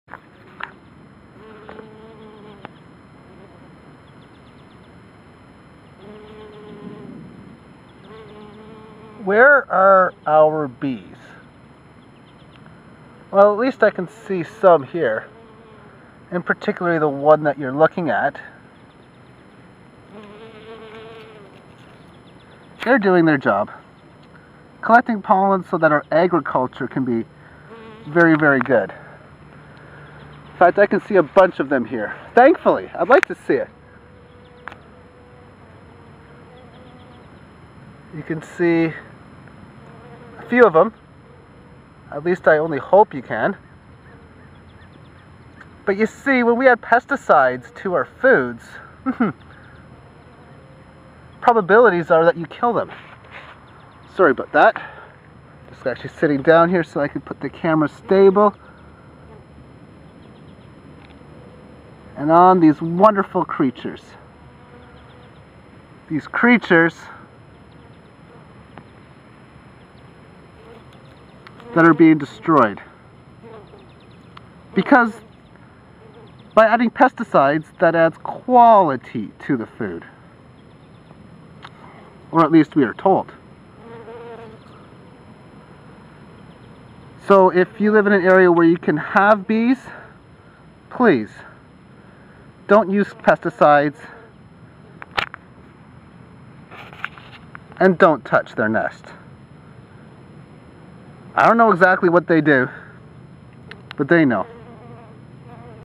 Honey Bees - They are so useful and big industry destroys them
Colony Collapse Disorder is real and when you go to your grocery store to buy veggies, they add quality due to pesticides in the food at the expense of our honey bees

Sarcastically speaking - thank you for work in spraying our plants